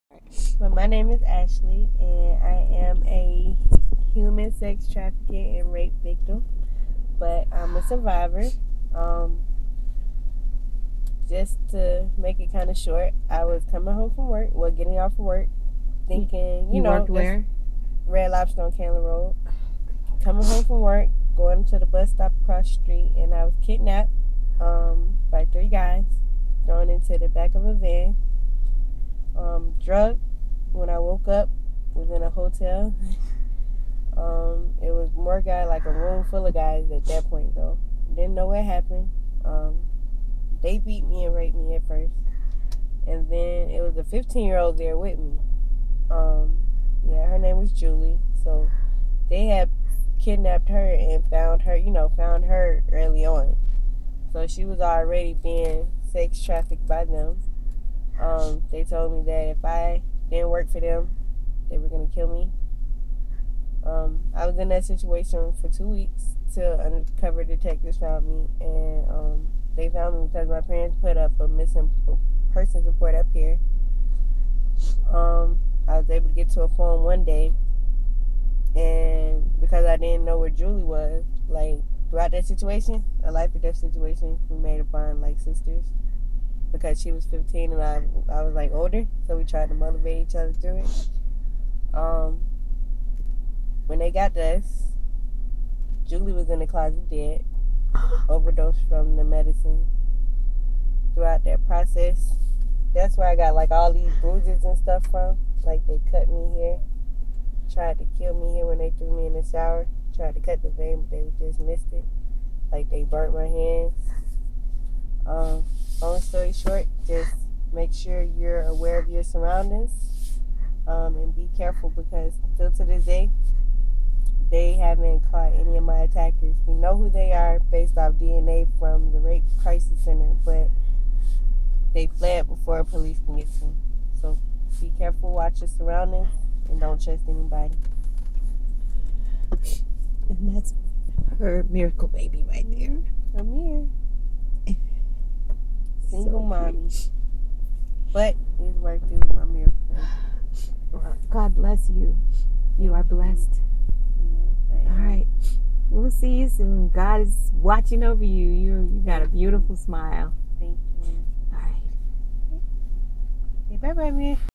Sex Trafficking Survivor in Atlanta
This young lady is a survivor of sex trafficking and her two month old baby is a product of the assault. A very touching story but very inspirational to other young ladies out there. A must-watch video of survival.